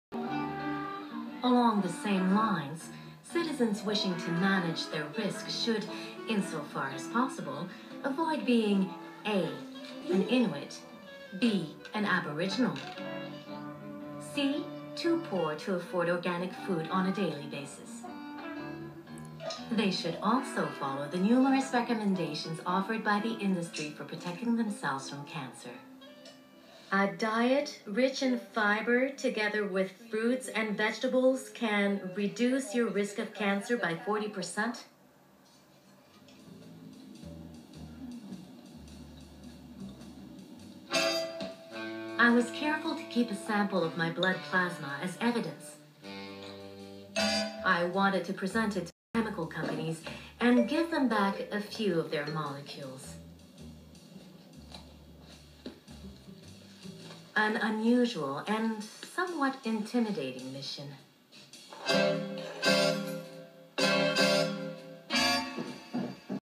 World-wide Pollution
Chemical pollution, fertilizers; the industry don't CARE! The poor, Inuit, and aboriginal will suffer!!!